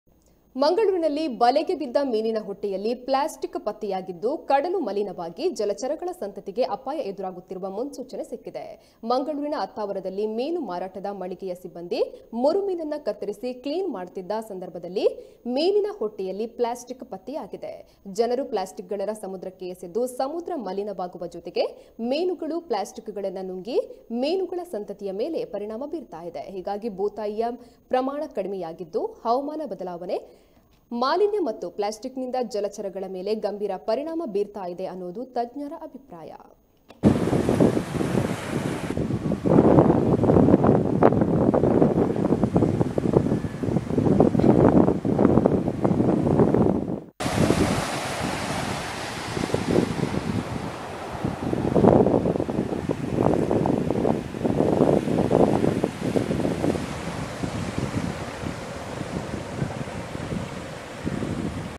ಬಲೆಗೆ ಬಿದ್ದ ಮೀನಿನ ಹೊಟ್ಟೆಯಲ್ಲಿ ಪ್ಲಾಸ್ಟಿಕ್ ಪತ್ತೆ ; ಕಡಲು ಮಲೀನವಾಗಿ ಜಲಚರದ ಸಂತತಿಗೆ ಎದುರಾದ ಅಪಾಯ
ಬಲೆಗೆ ಬಿದ್ದ ಮೀನಿನ ಹೊಟ್ಟೆಯಲ್ಲಿ ಪ್ಲಾಸ್ಟಿಕ್ ಪತ್ತೆ ; ಕಡಲು ಮಲೀನವಾಗಿ ಜಲಚರದ ಸಂತತಿಗೆ ಎದುರಾದ ಅಪಾಯ - ಕಹಳೆ ನ್ಯೂಸ್